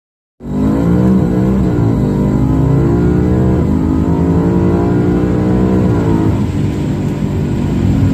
AUDI S4 B5 4.2 V8 TWIN TURBO first test new setup DRAGY onboard 8hp gearbox
8hp gearbox first test audi b5 platform .